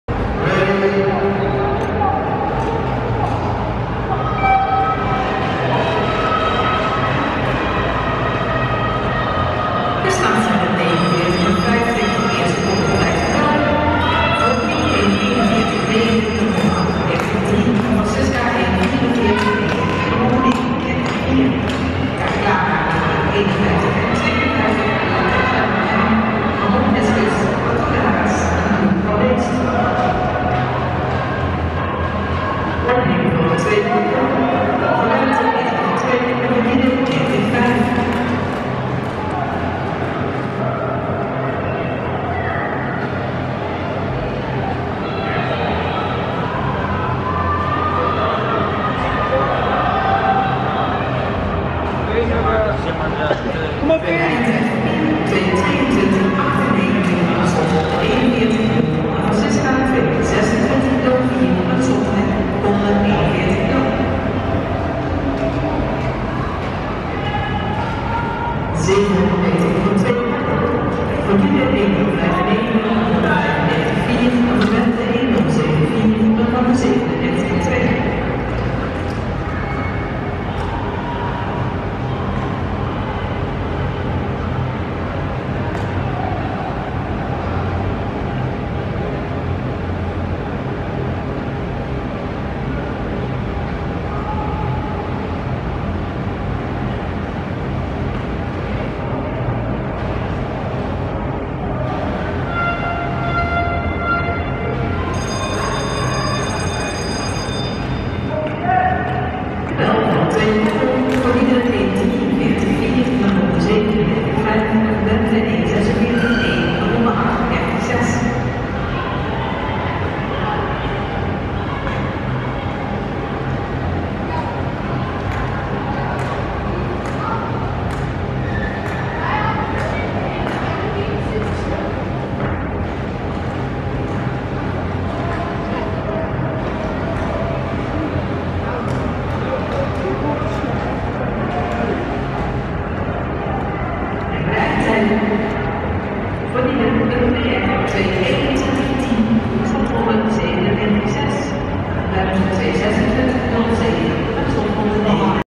Diede van Oorschot-Bente Slagter 1500 Eindhoven 20180113
Diede van Oorschot vs Bente Slagter
1500 meter
20180113, IJssportcentrum Eindhoven
Diede 2.21.10 (PR), Bente 2.26.07